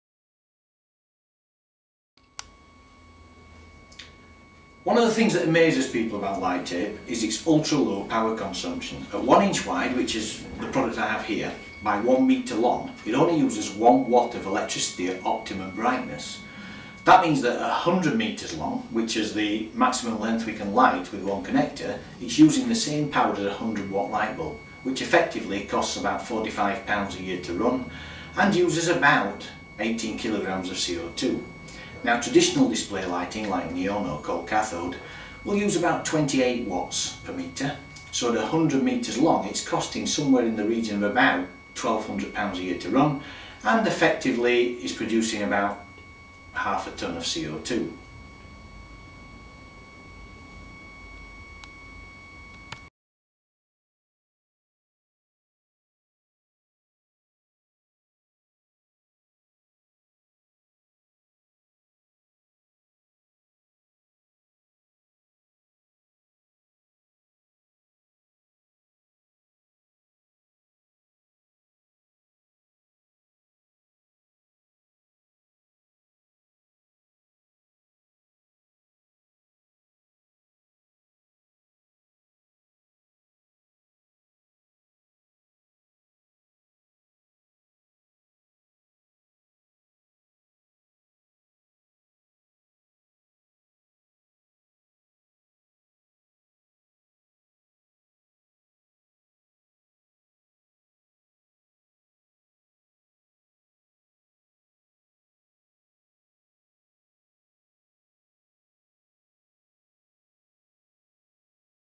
What is Light Tape? Part 1 Slim flexible low carbon lightin
Light Tape is a flat, thin, flexible, environmentally friendly alternative to traditional display lighting like neon and cold cathode, using no glass, no gas and no mercury.